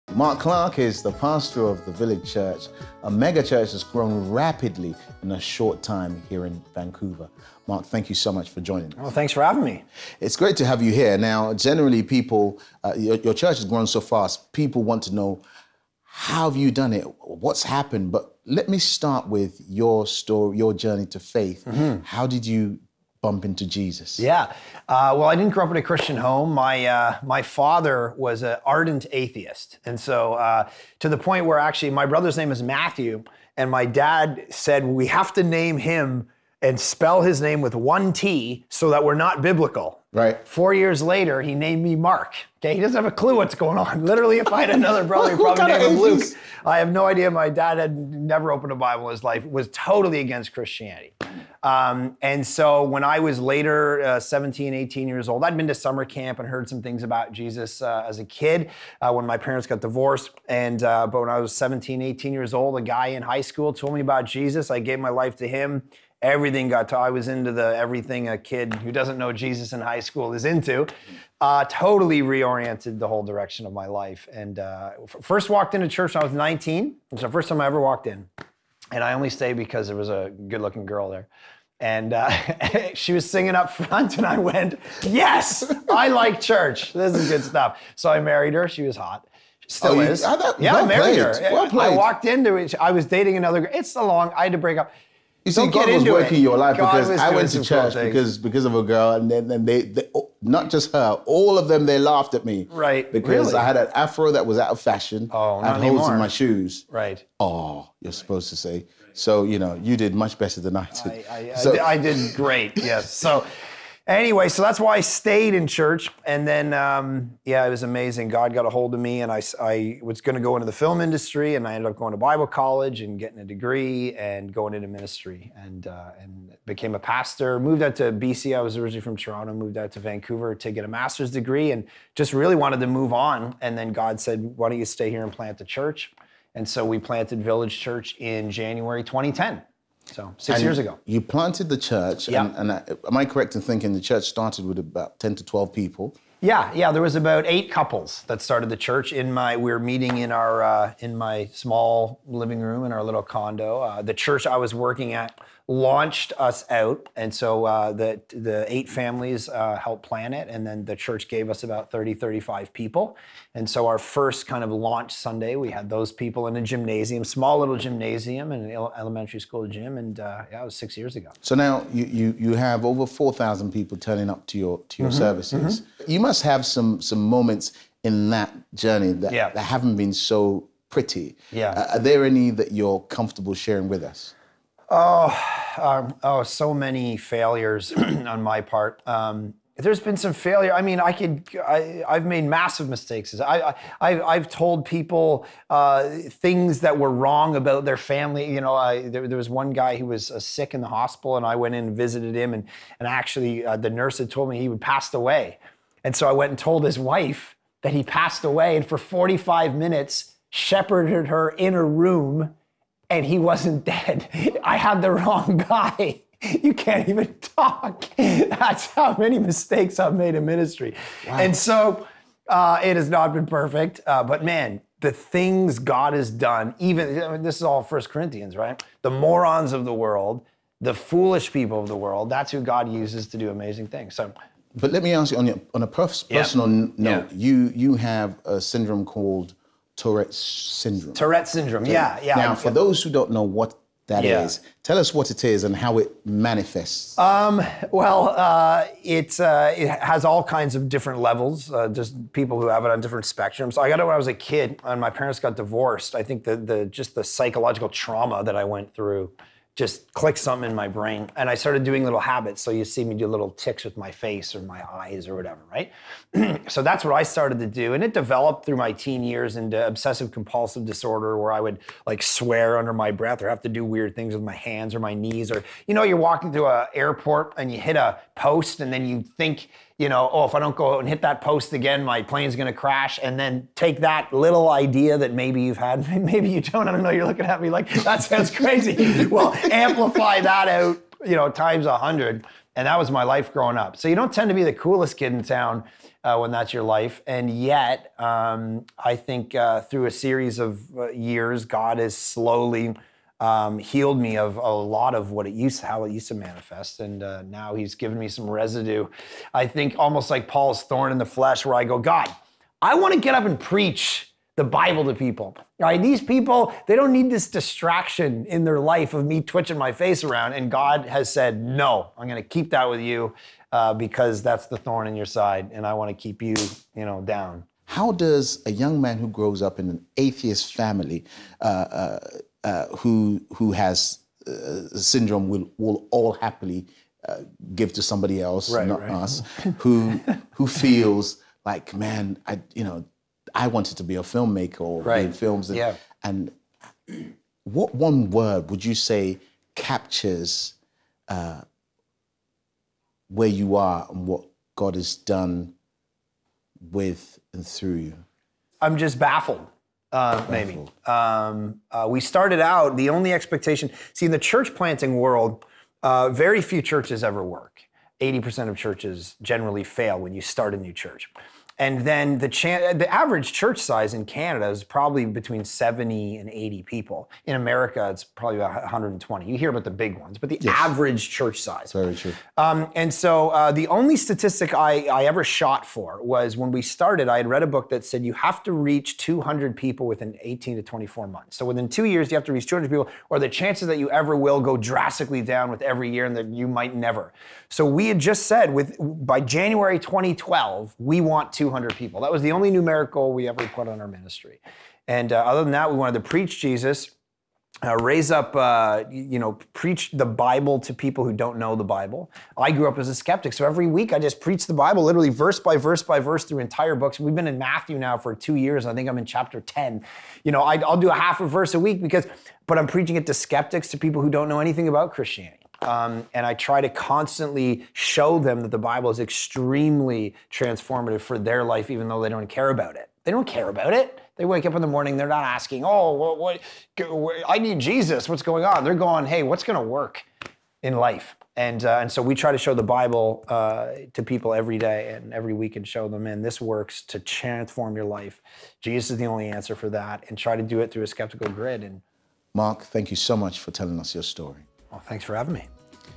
Interview | Pastor Mark Clark
The pastor shares how he grew his congregation from 12 to 5,000 in 5 years and how he does it with tourette's syndrome.